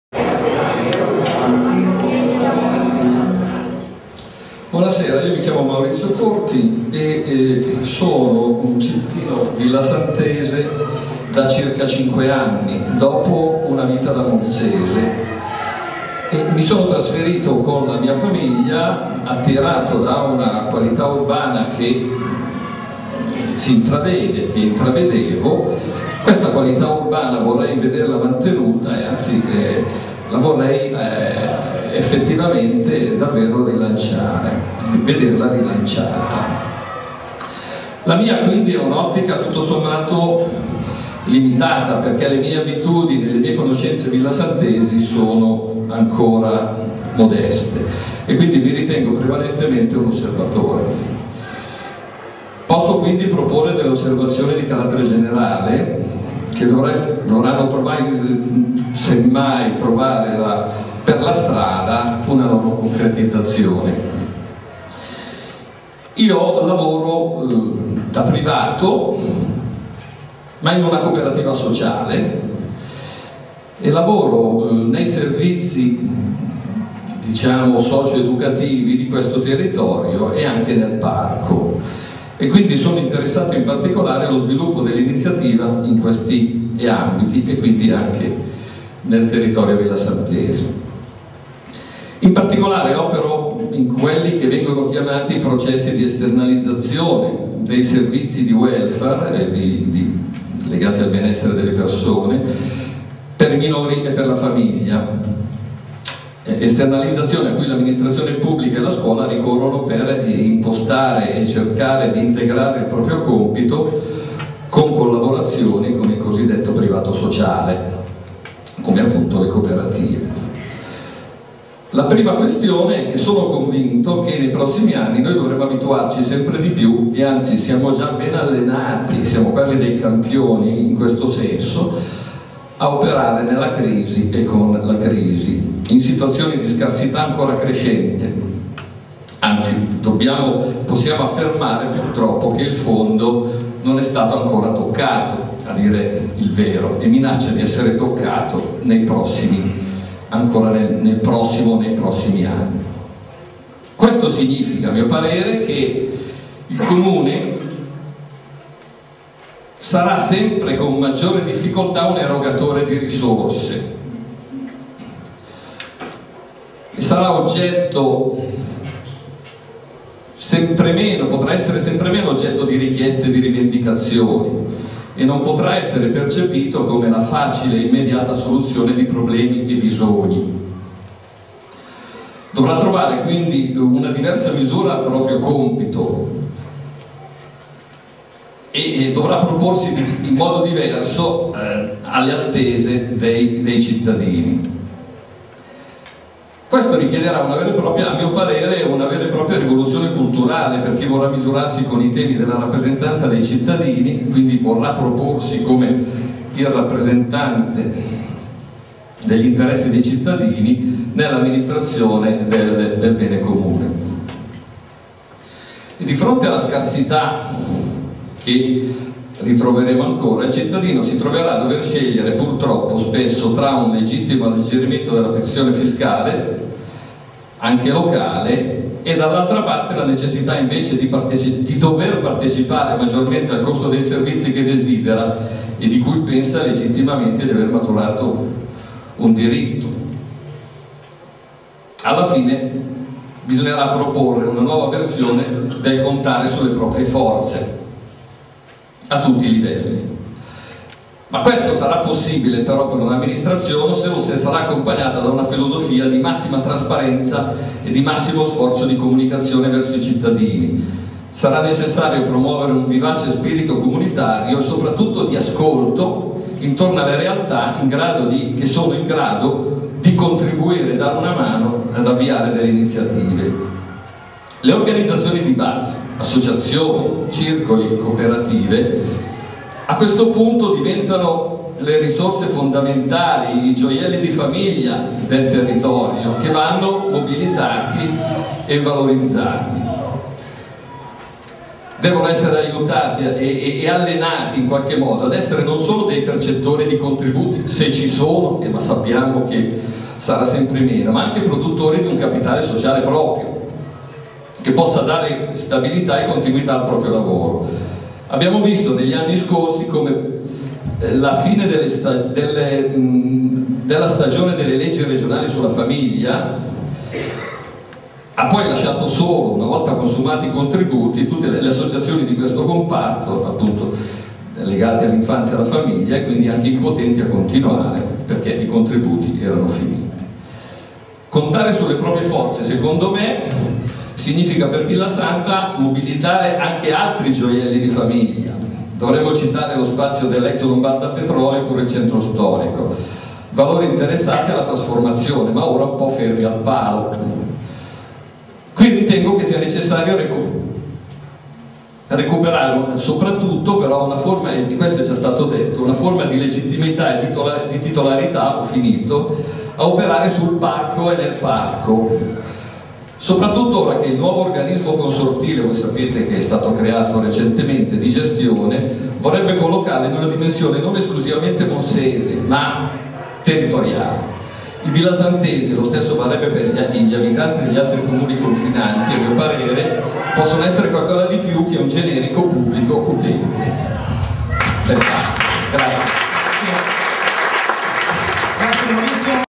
Il Punto delle Idee - Maurizio Corti
Il Punto delle Idee. La manifestazione organizzata da "Il Punto di Villasanta" che venerdi 10 maggio 2013 ha dato la parola ai cittadini per costruire il futuro della città.